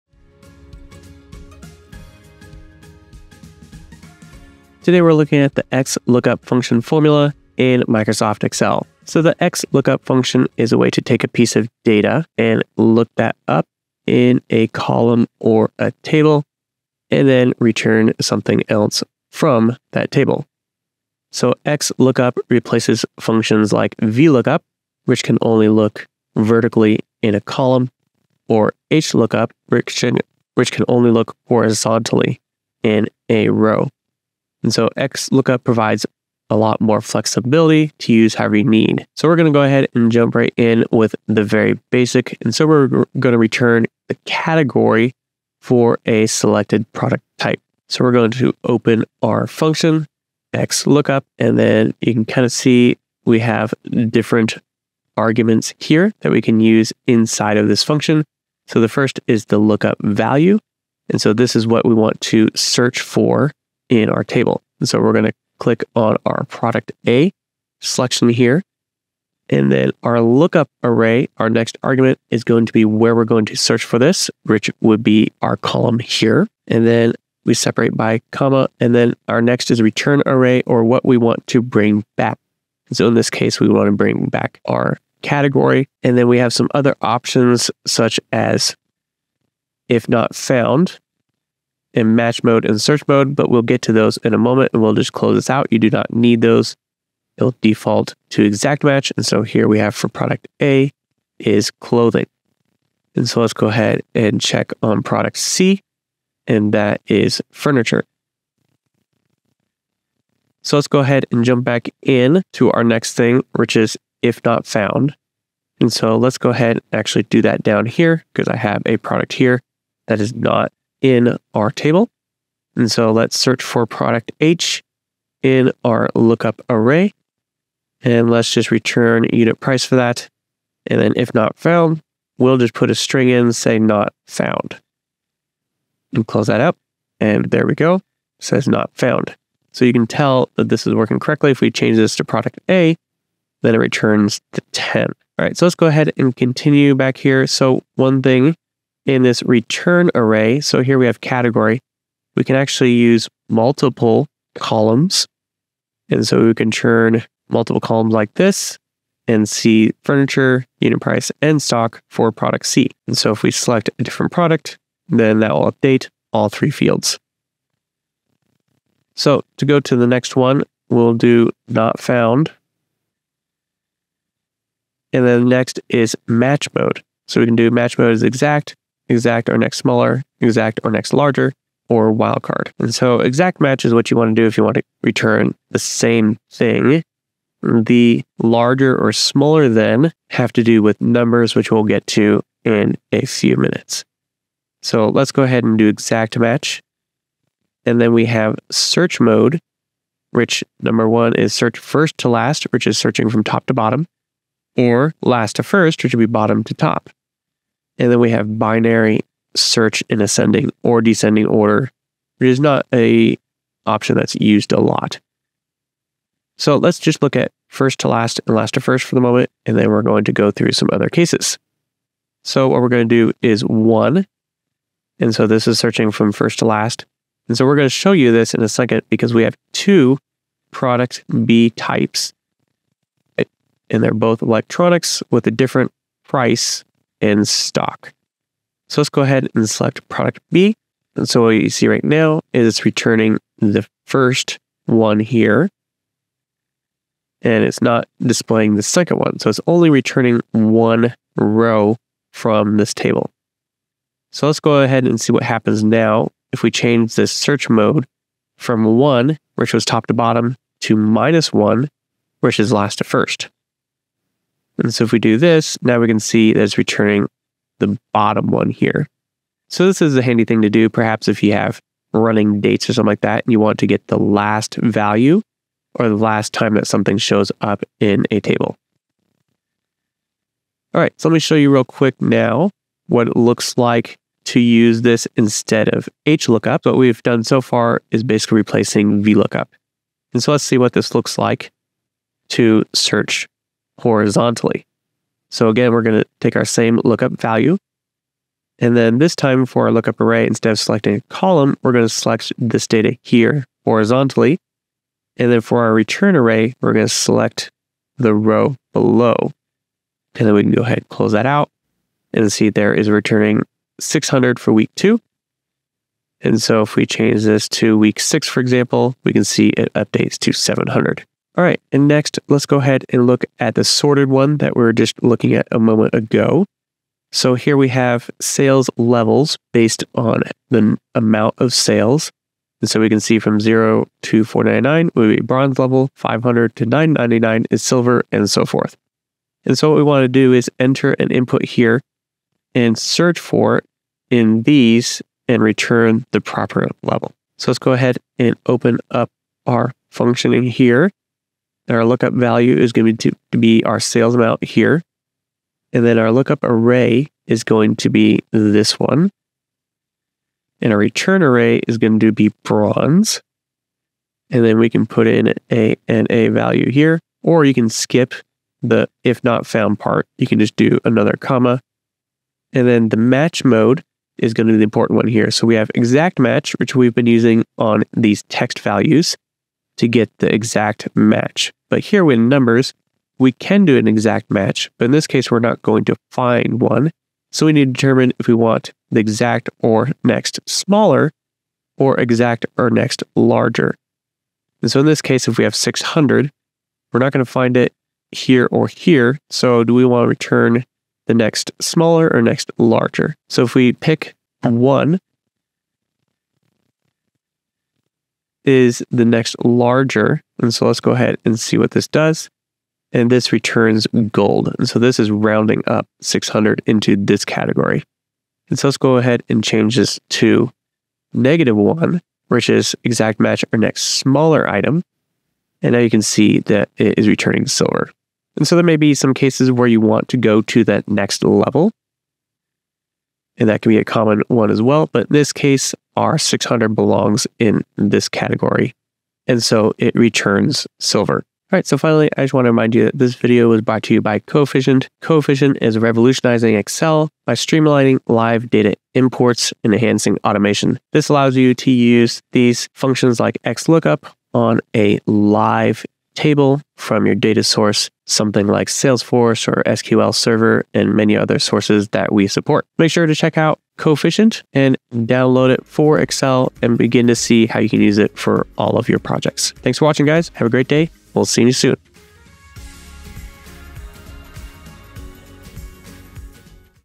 How to Use the XLOOKUP Function in Excel
Learn to use the XLOOKUP function in Excel. This is a complete walkthrough with different use cases that might apply to your business.

The XLOOKUP function searches a range or an array, and then returns the item corresponding to the first match it finds. If no match exists, then XLOOKUP can return the closest (approximate) match. *If omitted, XLOOKUP returns blank cells it finds in lookup_array.